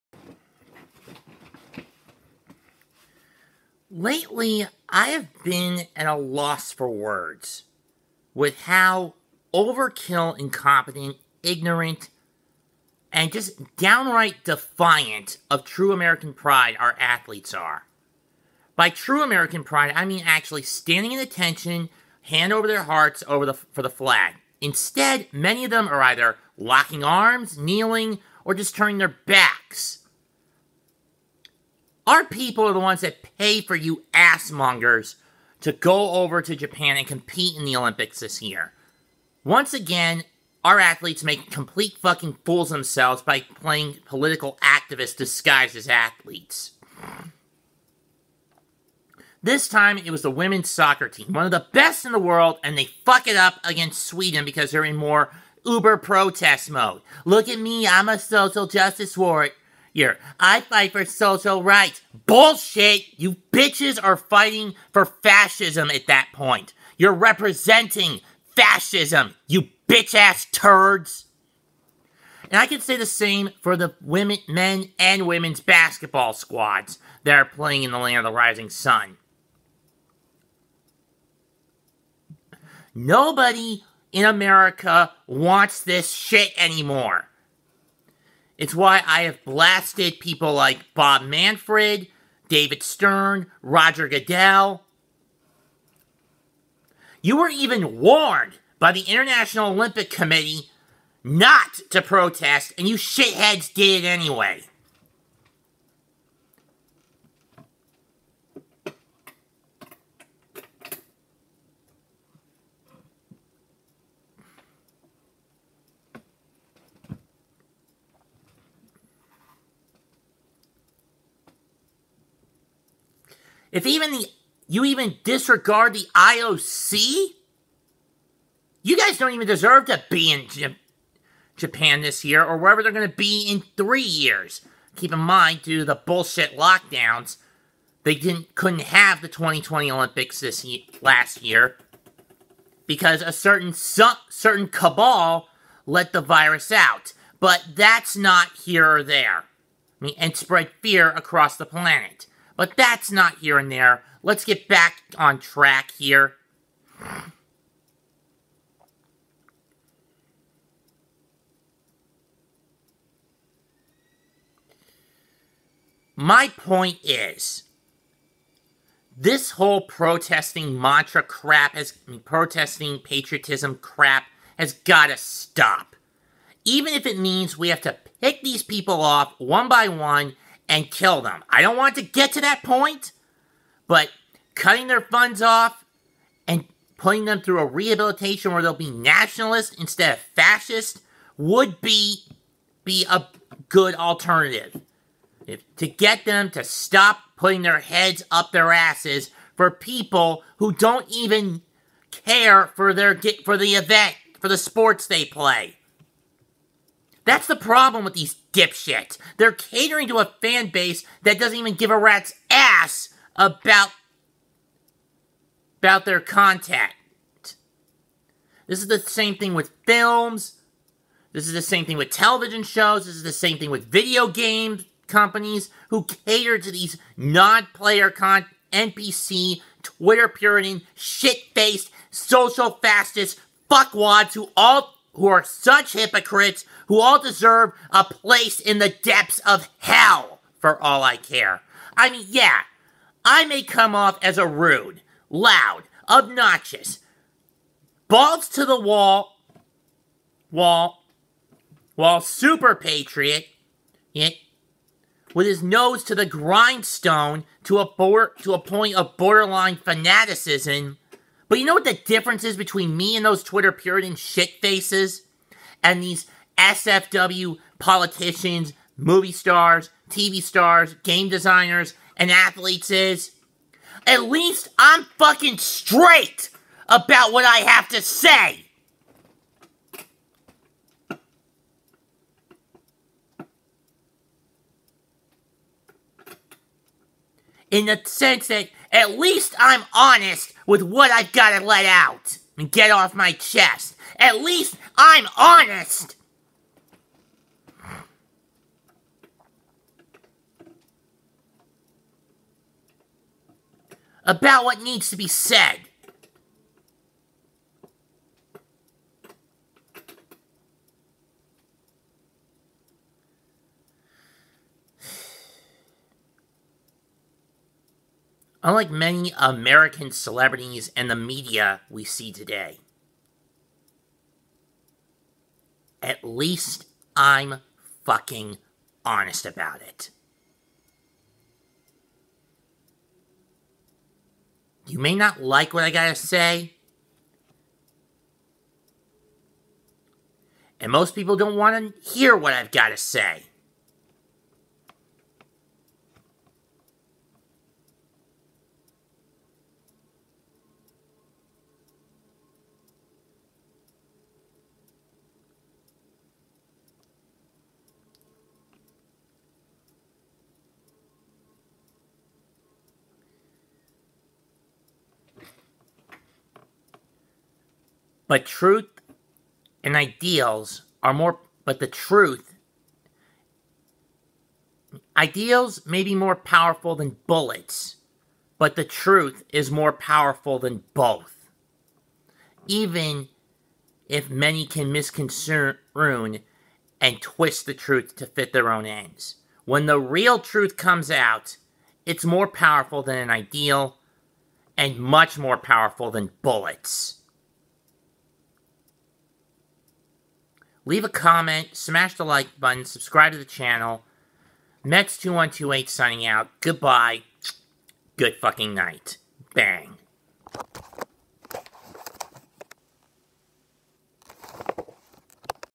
Rant against American athletes
After hearing how the US women’s soccer teamed joined the both US basketball squads in protesting which cost them a game against Sweden. Mets decides to sound off on these Fascist hypocrites. It may come off as mean but Mets doesn’t care and believe him when he says this is going to get ugly.